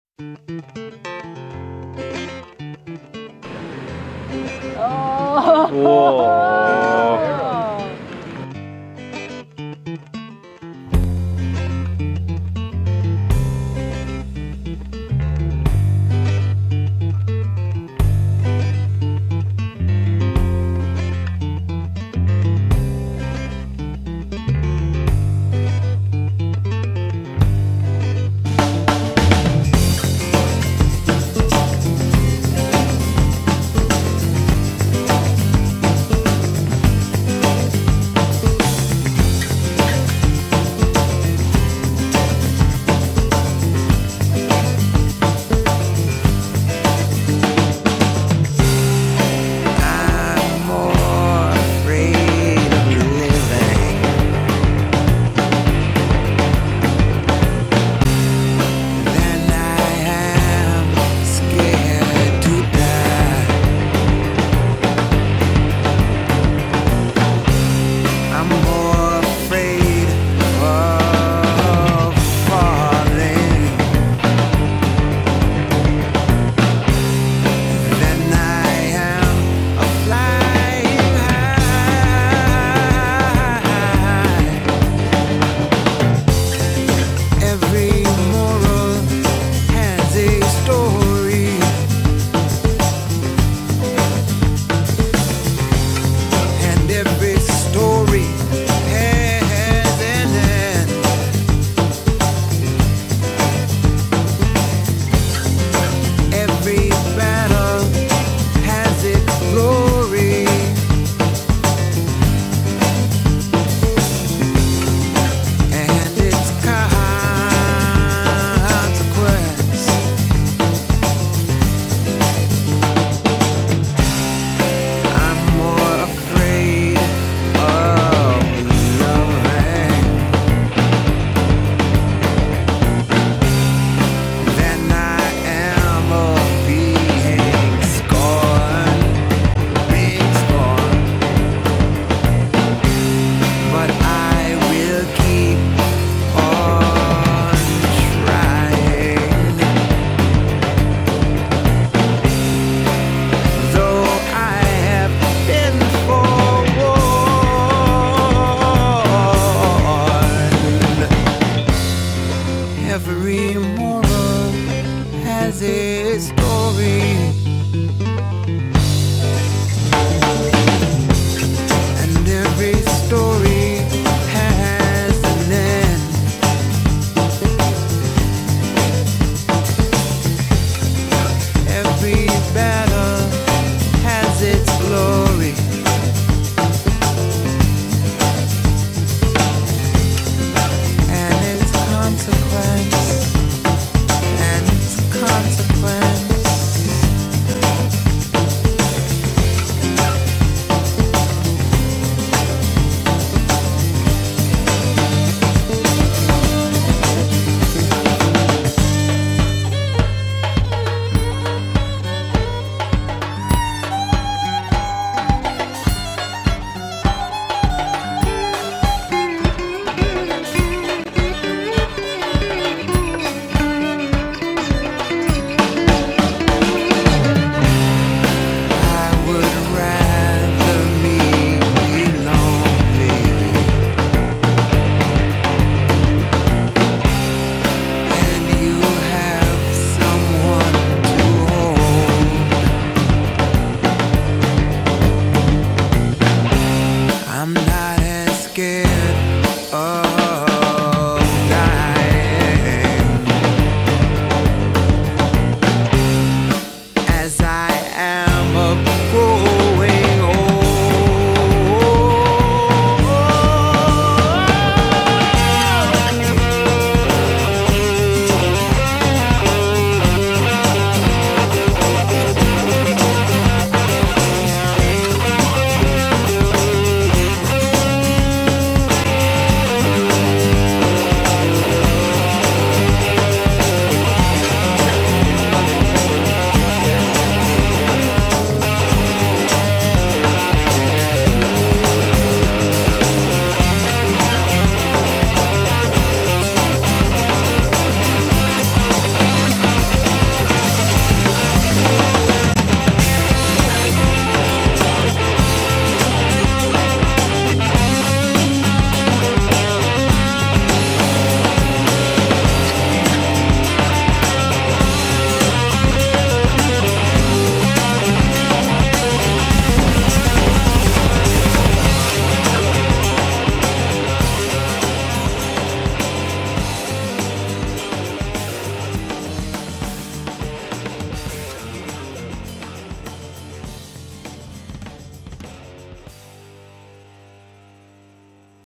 McDonald Family Adventure in Nicaragua
Lindsay had been on a couple of retreats and came back with her brother and his fiance to show them what surfing is all about.